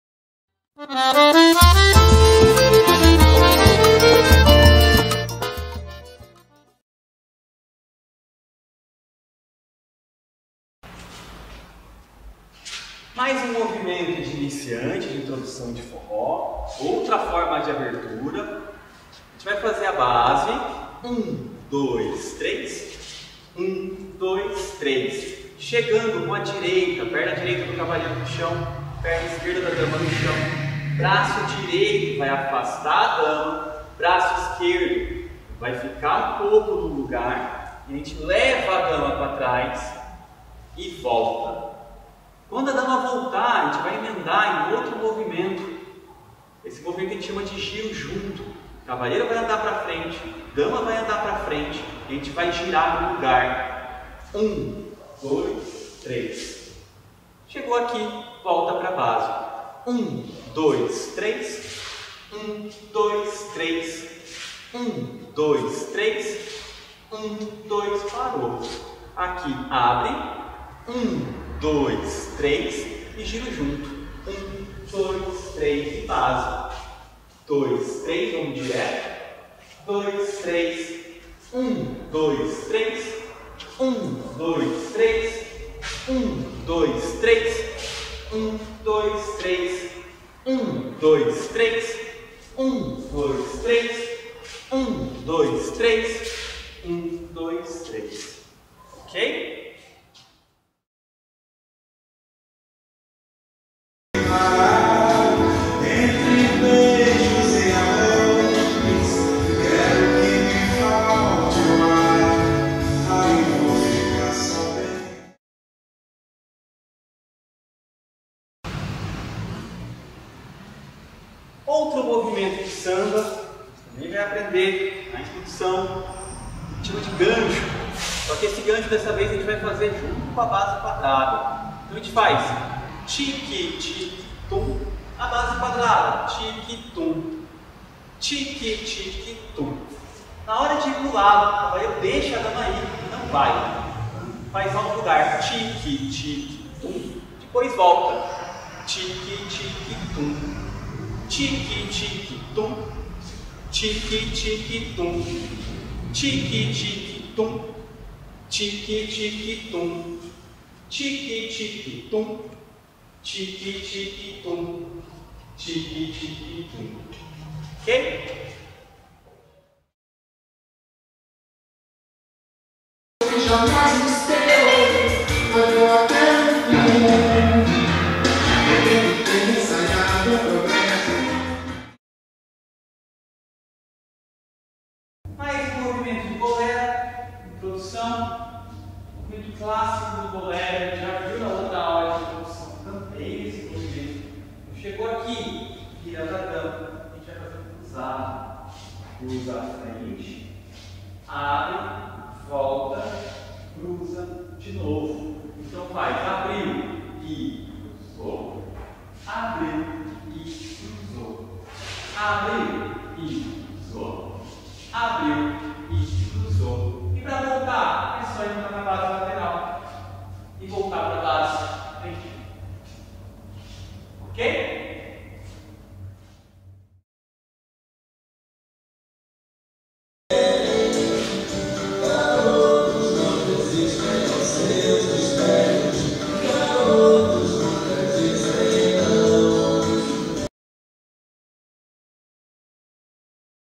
Aulas Diárias - #240 - 2020.12 - 16.28 - Quarta-feira - Forró, Samba e Bolero Introdução
Nossa terceira aula de Forró, Samba e Bolero Introdução no mês de Dezembro. Tudo que você precisa saber para dançar direitinho no salão

Gostou das aulas, nos apoie e garanta que teremos aulas toda semana ou todos os dias para sempre :D